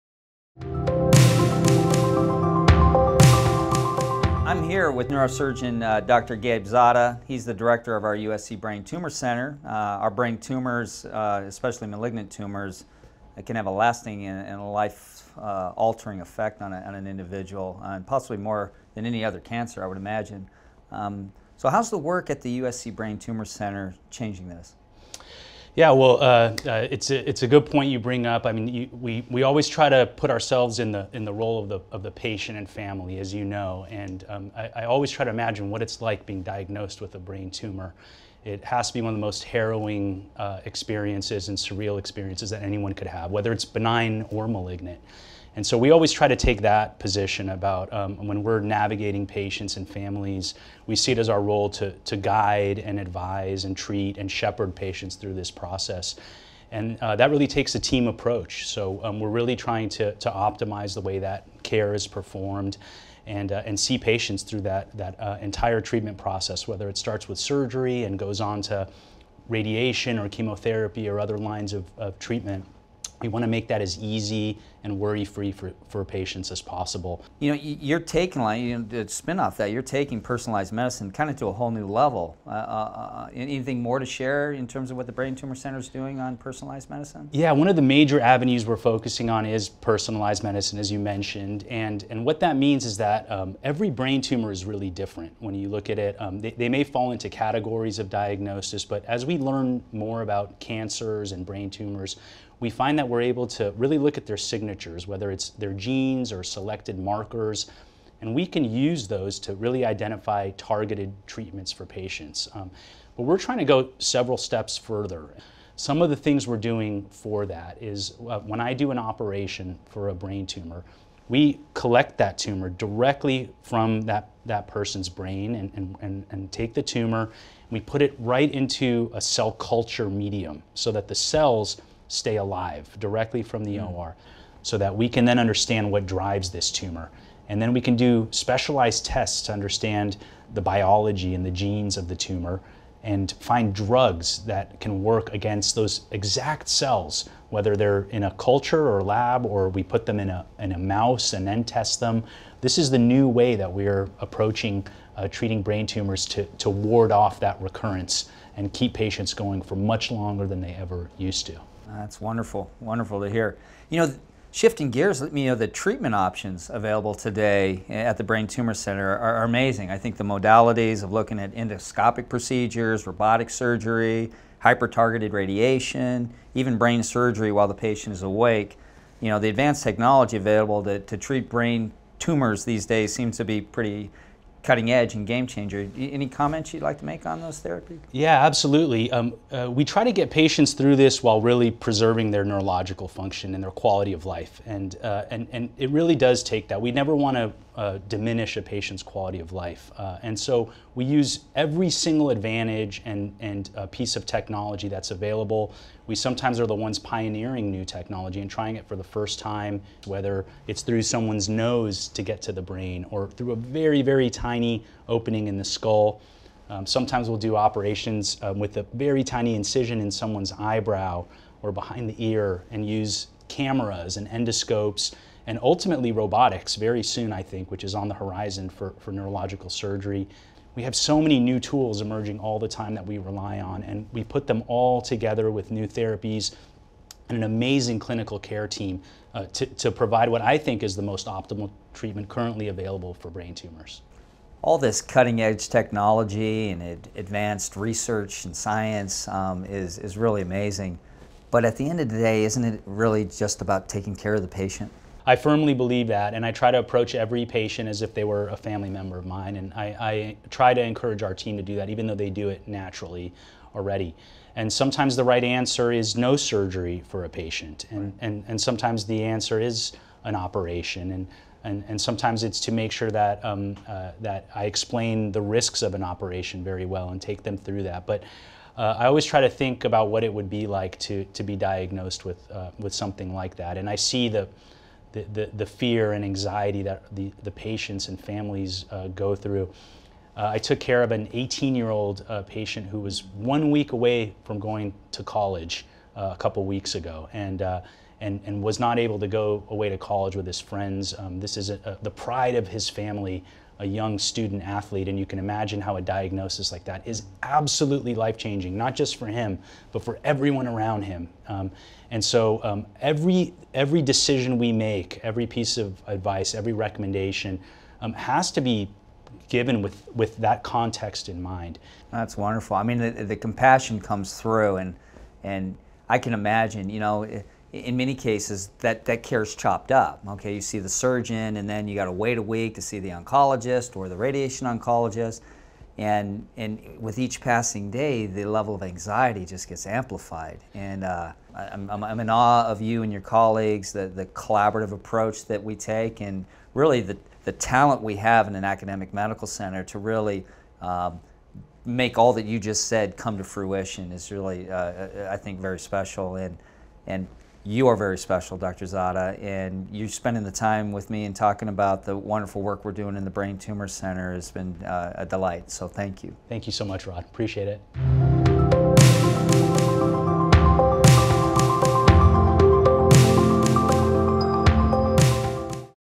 Dr. Gabriel Zada Discusses the Importance of Personalized Medicine for Brain Tumor Care
Rod Hanners, CEO of Keck Medicine of USC, speaks with Gabriel Zada, MD, director of the USC Brain Tumor Center, about how his team uses an interdisciplinary approach with personalized medicine and cutting-edge technology to tailor treatment plans for each patient.